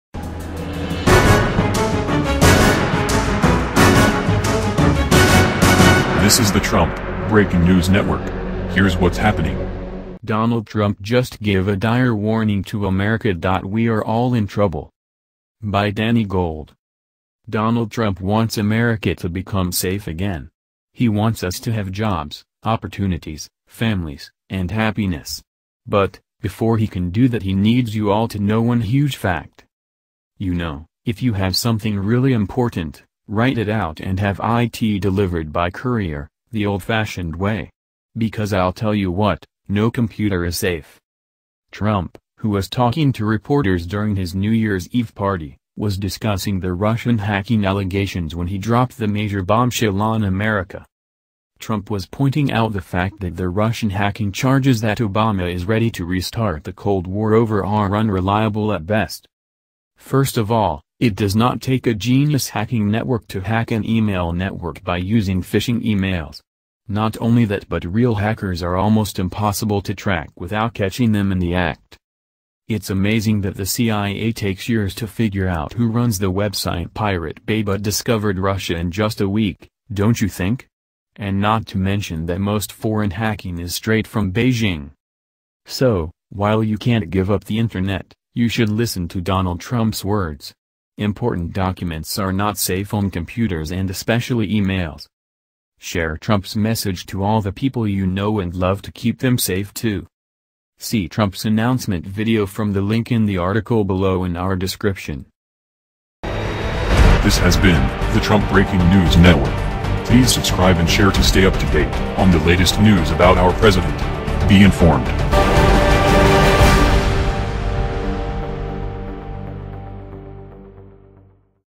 Donald Trump Just Gave a DIRE Warning to America…We are ALL in Trouble!
Trump Breaking News Network - Donald Trump Just Gave a DIRE Warning to America…We are ALL in Trouble!

Donald Trump wants America to become safe again. He wants us to have jobs, opportunities, families, and happiness. But, before he can do that he needs you all to know one HUGE Fact…

“YOU KNOW, IF YOU HAVE SOMETHING REALLY IMPORTANT, WRITE IT OUT AND HAVE IT DELIVERED BY COURIER, THE OLD-FASHIONED WAY. BECAUSE I’LL TELL YOU WHAT: NO COMPUTER IS SAFE.“

Trump, who was talking to reporters during his New Years Eve party, was discussing the Russian hacking allegations when he dropped the major bombshell on America.

Trump was pointing out the fact that the Russian hacking charges that Obama is ready to re-start the Cold War over are unreliable at best.

First of all, it does not take a genius hacking network to hack an email network by using phishing emails. Not only that but REAL hackers are almost impossible to track without catching them in the act.

It’s amazing that the CIA takes YEARS to figure out who runs the website Pirate Bay but discovered Russia in just a week, don’t you think? And not to mention that MOST foreign hacking is straight from Beijing.

So, while you can’t give up the internet, you should listen to Donald Trump’s words. Important documents are NOT safe on computers and especially emails.

Share Trump’s message to all the people you know and love to keep them safe too.

See Trump's announcement video from the link in the article below in our description.